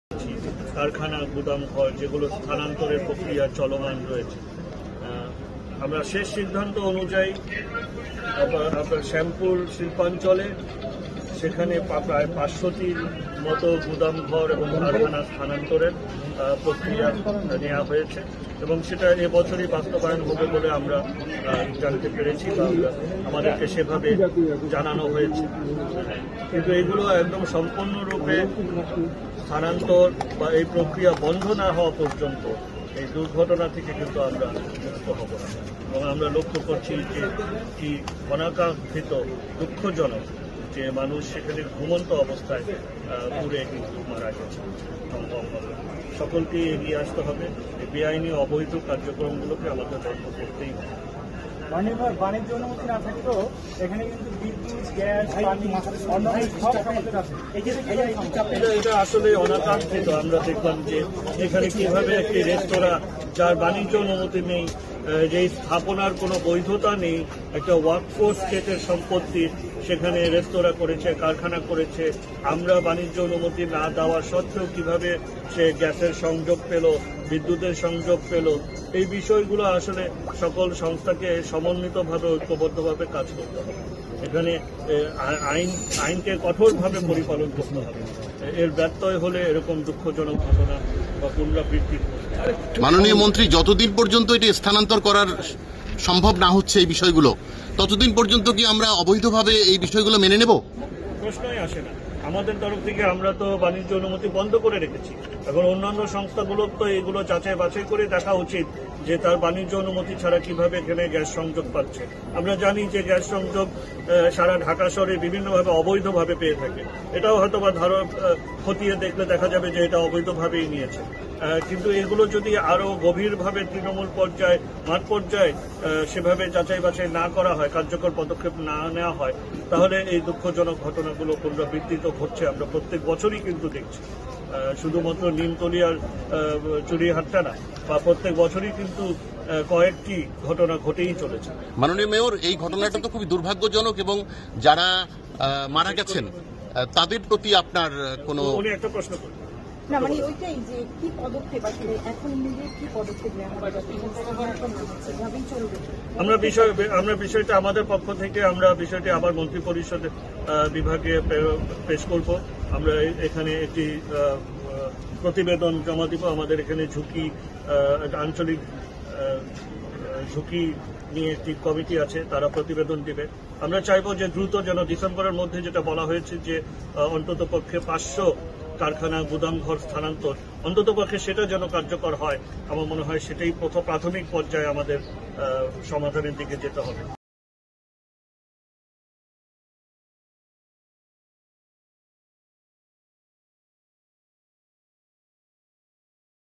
ডিসেম্বরের মধ্যে ৫০০ অবৈধ কারখানা সরিয়ে নেওয়ার ঘোষণা মেয়রের
worldwide.